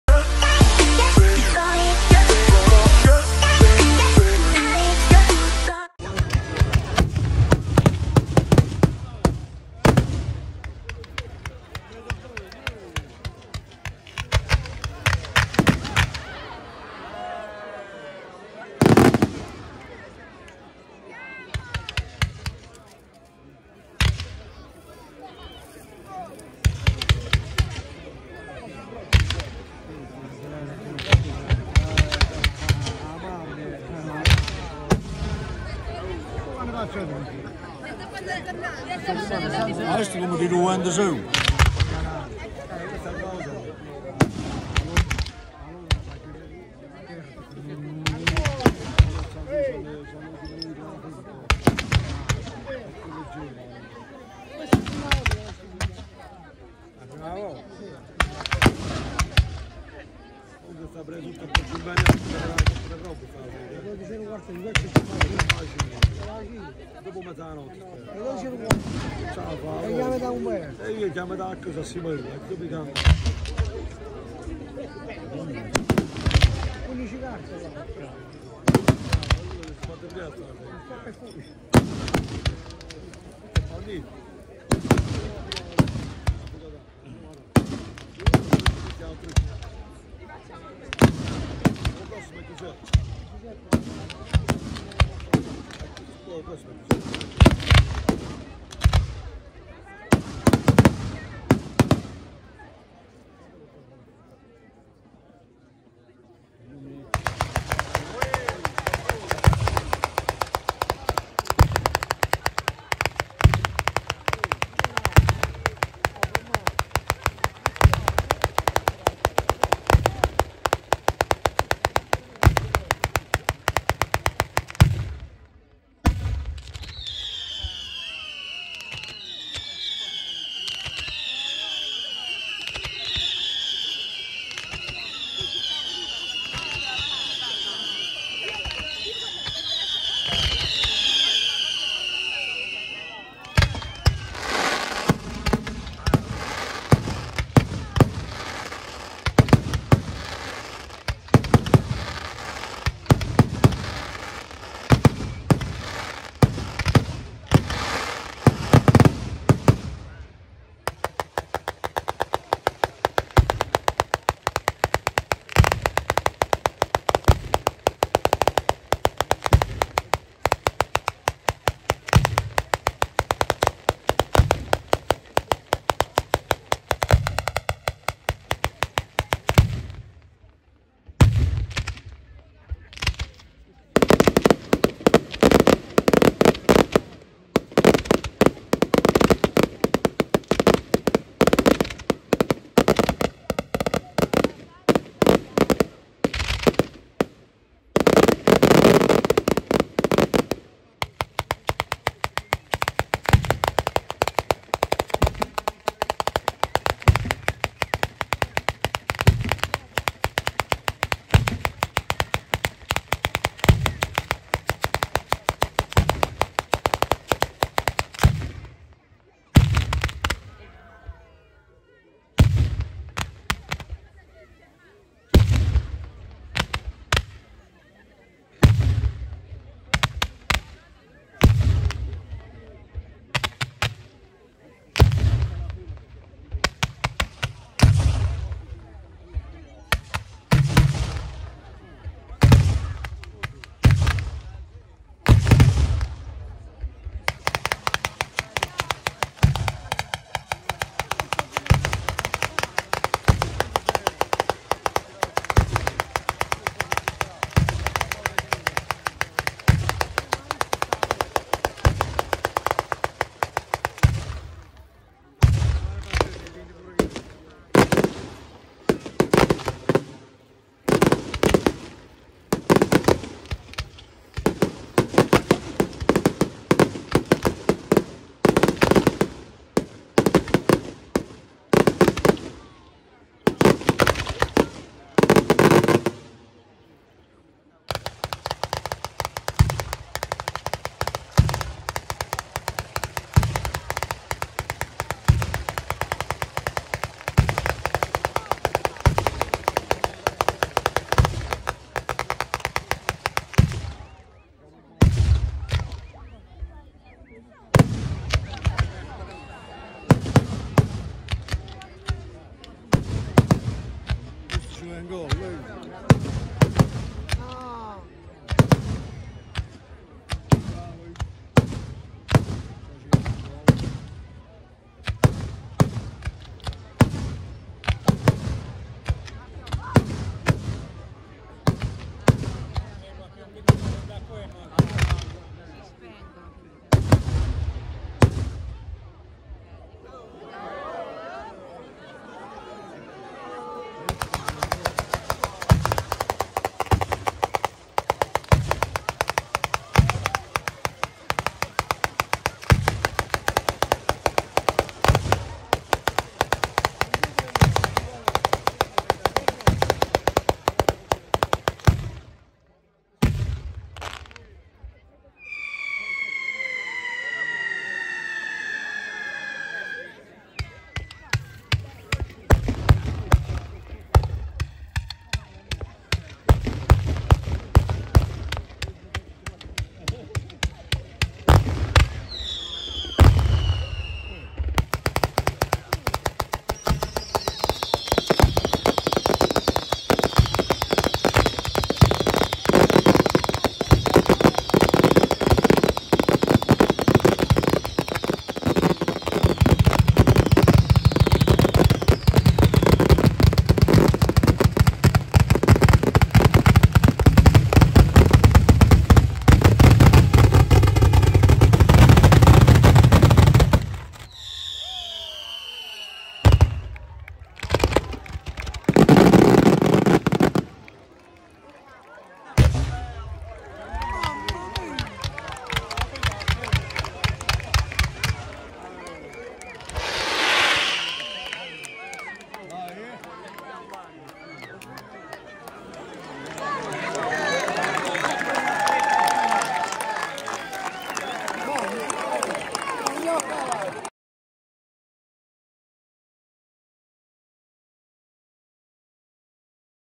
Stornarella (Fg) I Fuochi di San Pio 2023 - NUOVA PIRODAUNIA - Spettacolo Serale
Stornarella (Fg) 08 Ottobre 2023
I Fuochi di San Pio - 7° Edizione
Festival dei Fuochi D'artificio - Grandiosa Batteria
NUOVA PIRODAUNIA di Michele Altrui da San Severo (Fg)